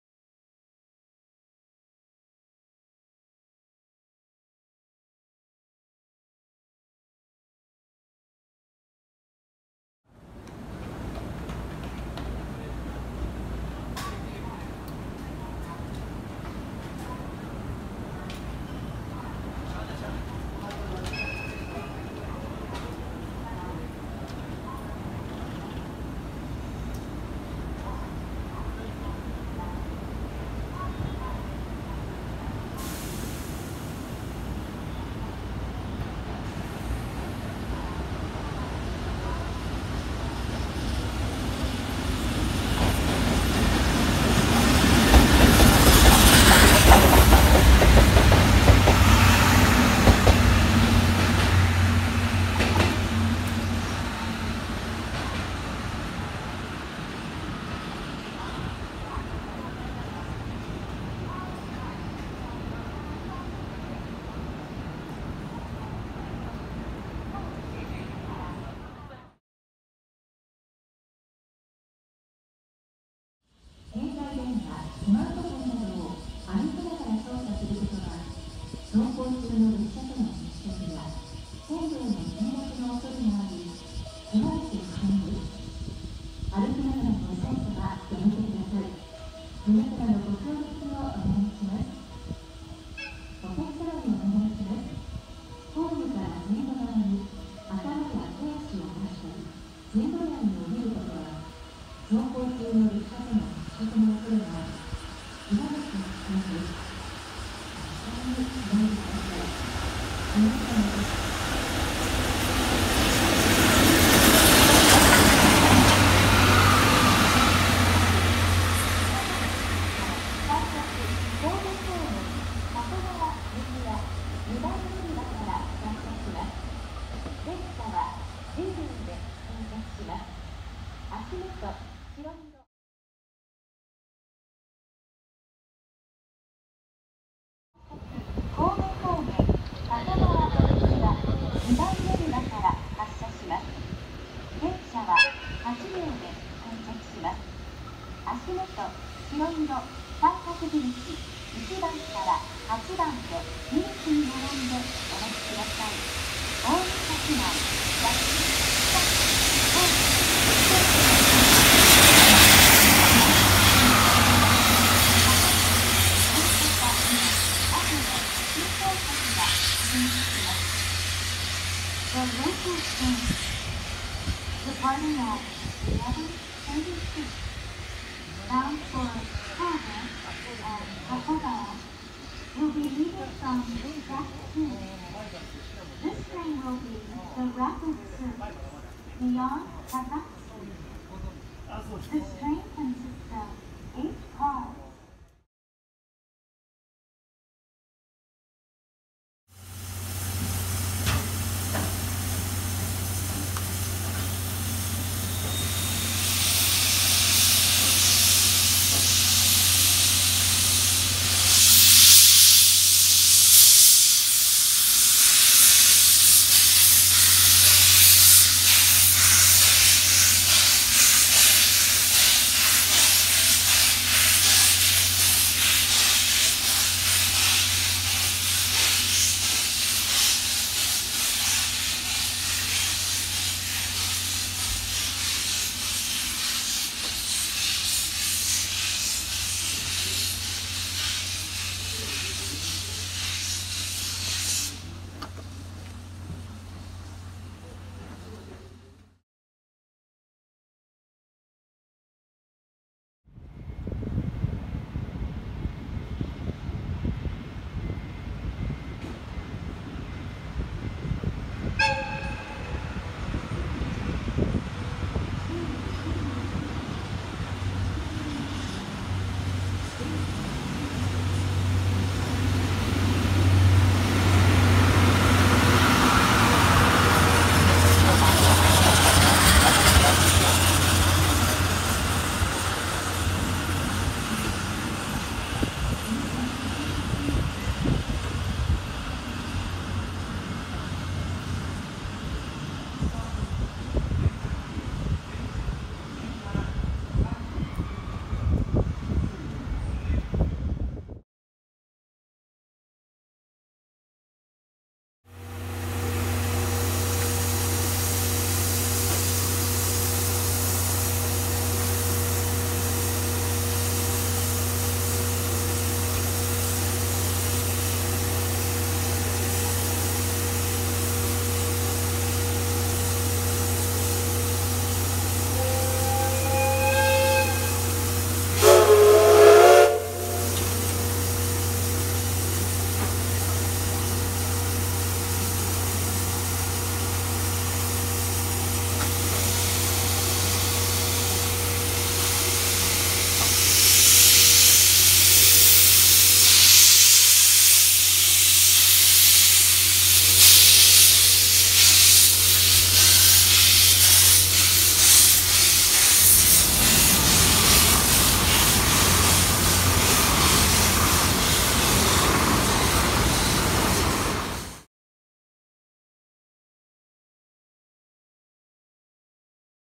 【SLデゴイチ‼️】DD51–200北陸線試運転送り込み・返却 @膳所・草津・守山・能登川・彦根にて
2023年7月28日（金）撮影

※北陸線試運転は都合により、行けず…😢